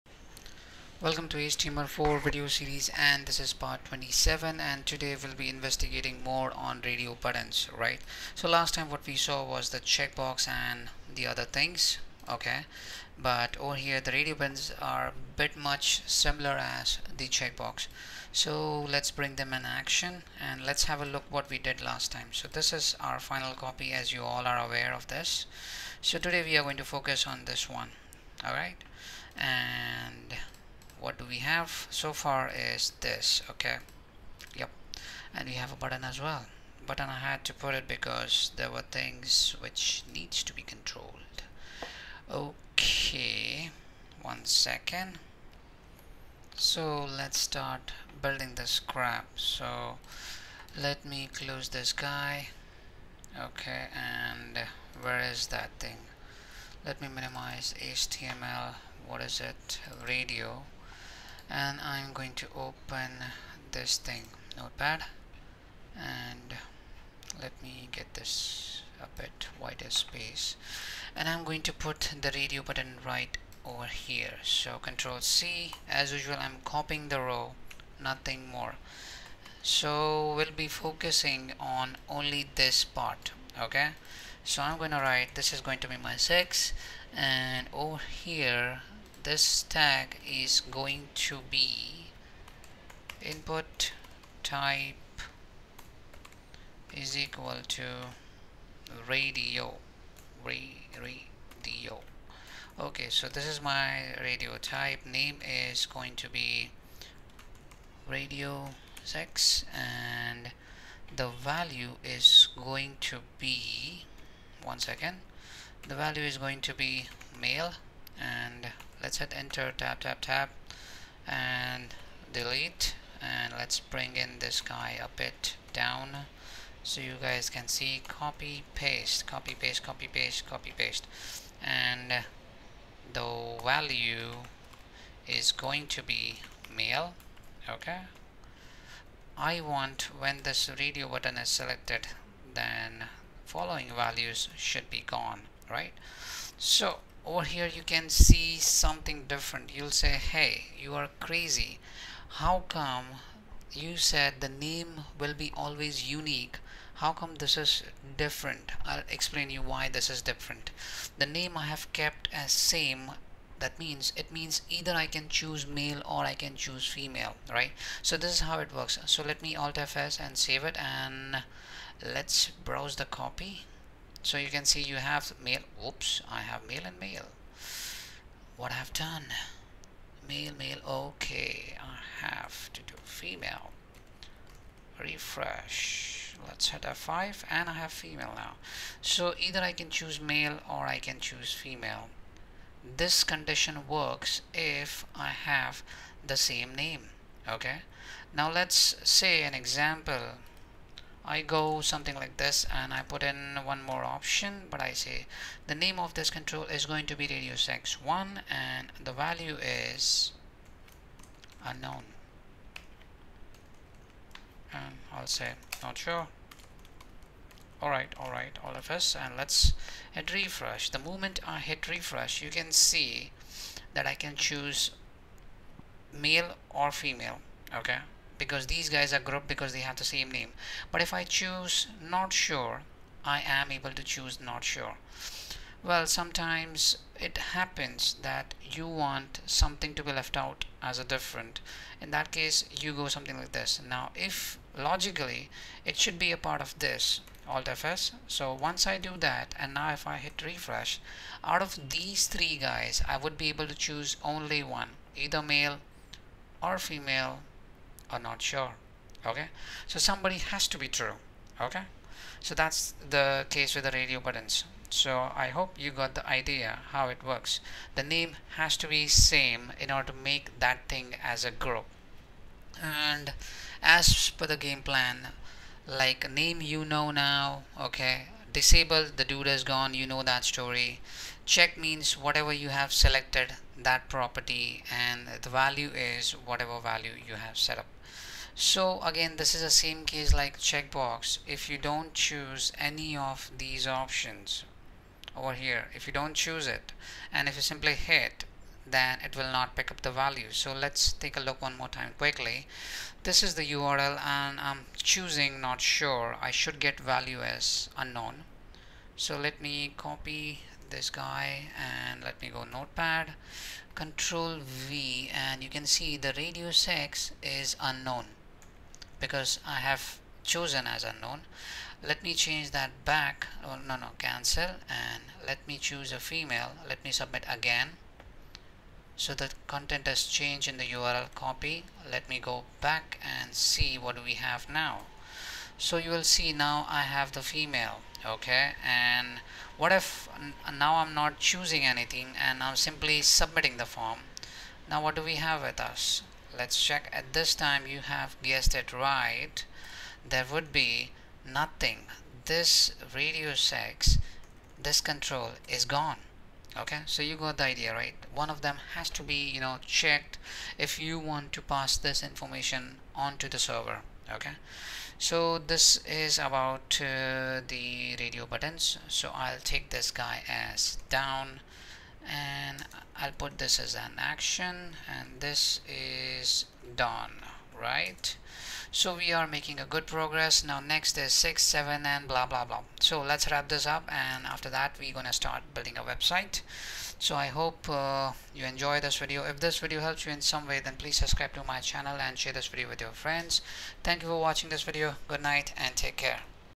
input type radio in html - HTML Tutorial for Beginners Part 27
This video explains how to create radio buttons and how to use them in html form.